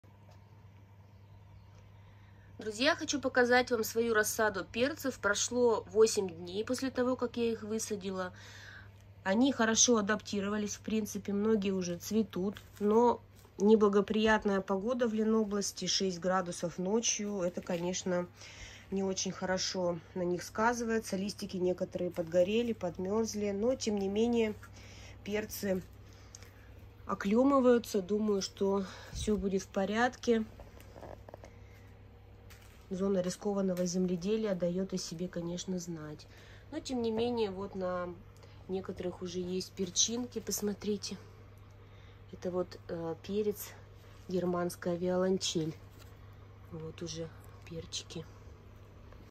8 дней после высадки перцев в теплицу
Выращиваю в Ленобласти